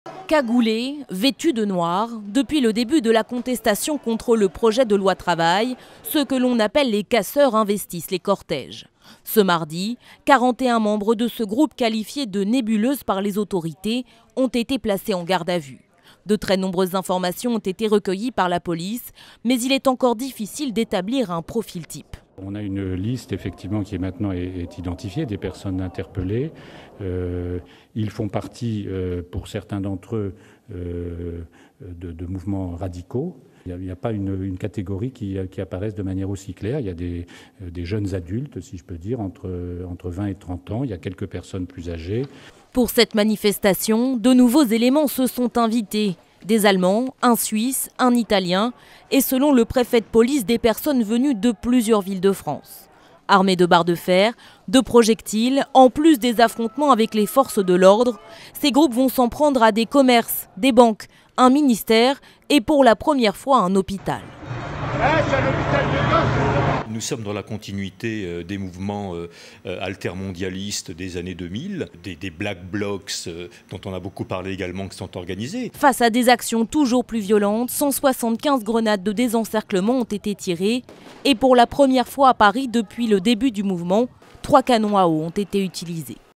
Manifestation contre la loi travail : qui sont les casseurs ?
41 personnes ont été placées en garde à vue mardi 14 juin, après les débordements en marge de la manifestation contre la loi travail. Vêtues de noires, elles font partie d'un groupe qualifié de "nébuleuse" par les forces de l'ordre. Des jeunes adultes difficiles à identifier, qui viennent de France mais aussi de l'étranger. Hier, 175 grenades de désencerclement ont été tirées et trois canons à eau utilisés pour disperser les casseurs.